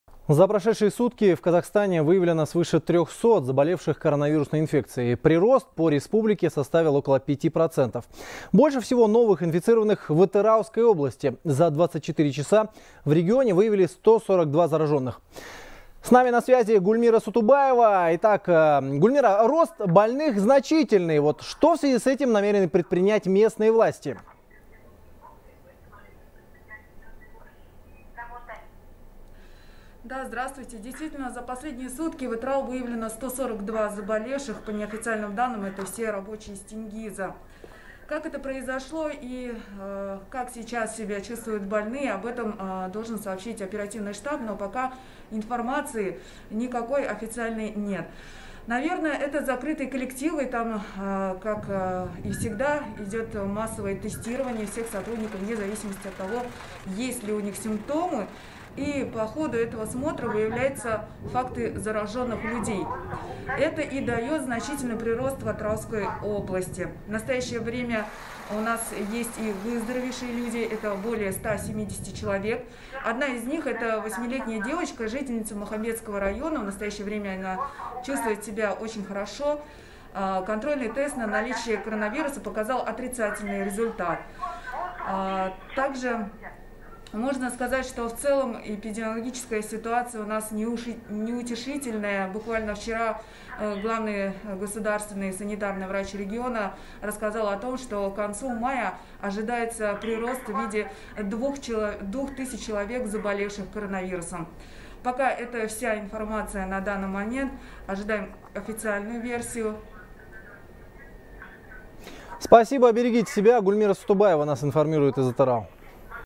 142 человека заразились COVID-19 за сутки в Атырауской области
За прошедшие сутки в Казахстане выявлено свыше трехсот заболевших коронавирусной инфекцией, передает корреспондент «Хабар 24».